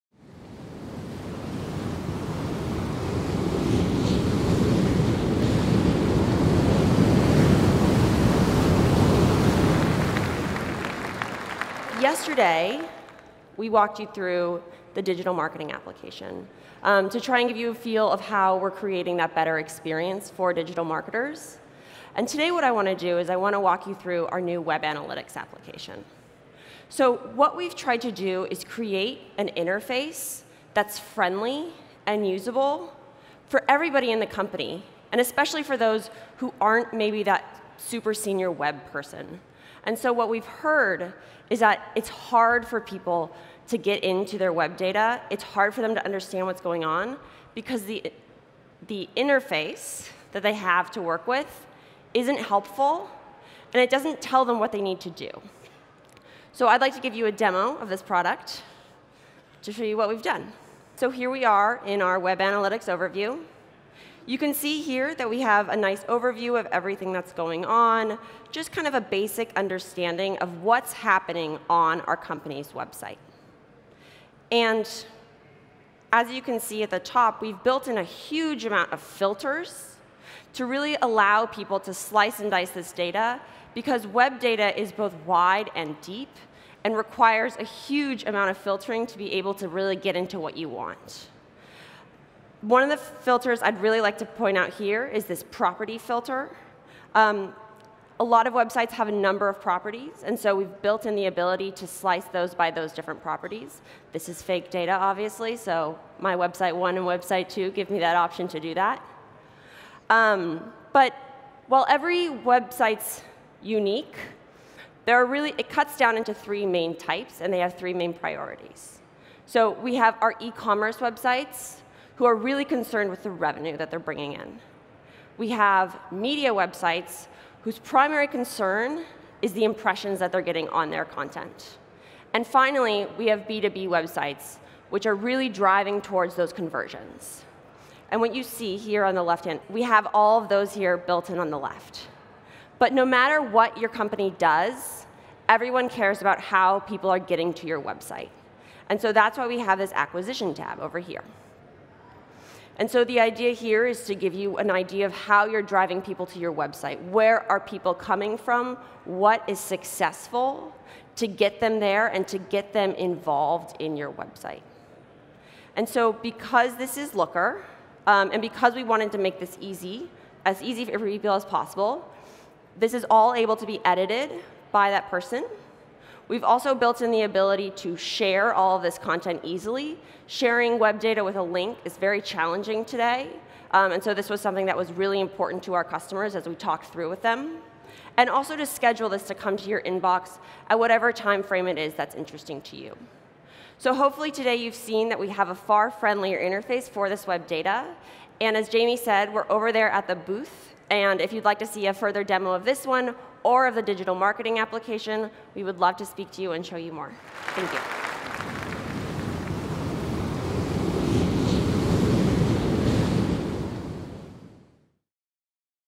JOIN 2018 - Elena Rowell: Looker 6 - Applications
Elena Rowell, Senior Product Marketing Manager at Looker, gives an update on Looker applications and demos the capability of the web analytics dashboard, available through Looker applications.